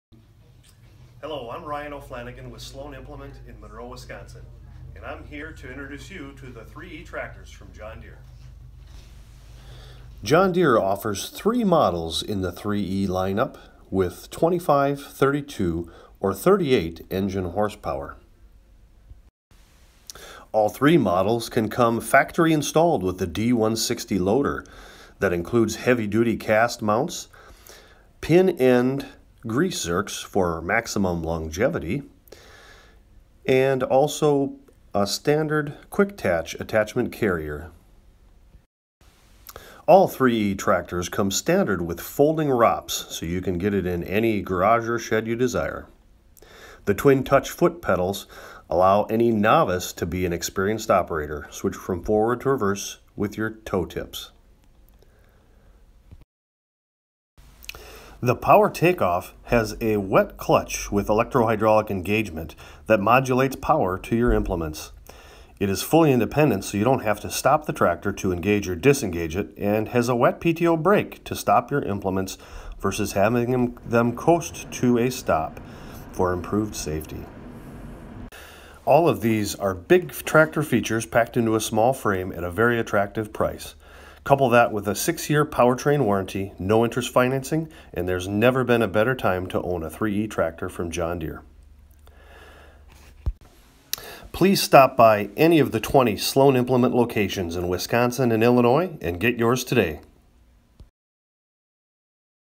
Monroe, WI: John Deere 3E Tractor Pitch Video
The Monroe, WI location of Sloan Implement submitted this video for week 3 of John Deere's 2017 National Sales Pitch video contest.  In this video, Monroe store manager Ryan O'Flanagan explains the features on the 3E tractor that make it the best value tractor in the 25 to 38 horsepower range.